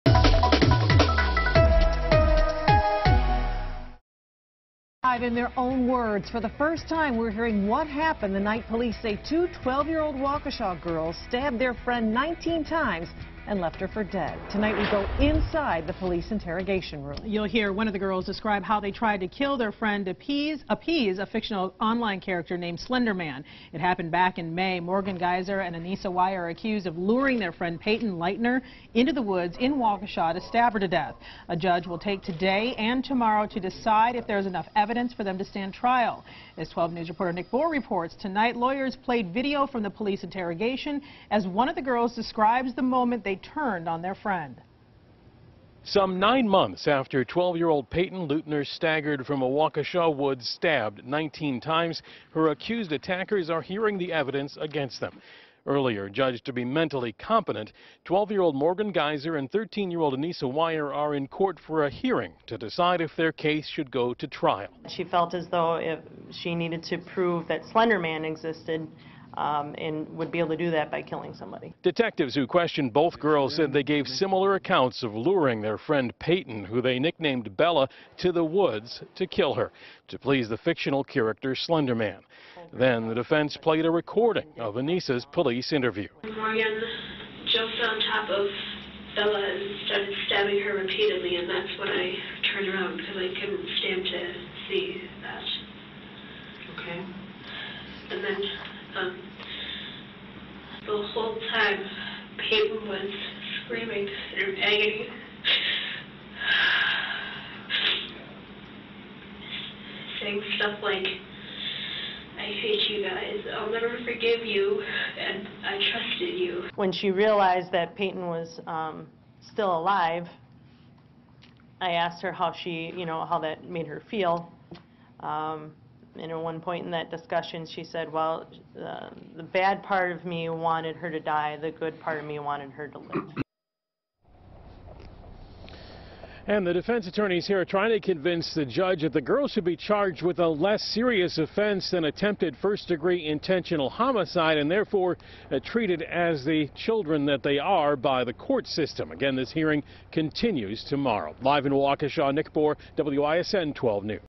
Stabbing suspect describes how she, another girl tried to please Slenderman
A suspect in the Waukesha stabbing case describes how she and another girl tried to kill their friend to please the fictional online character Slenderman.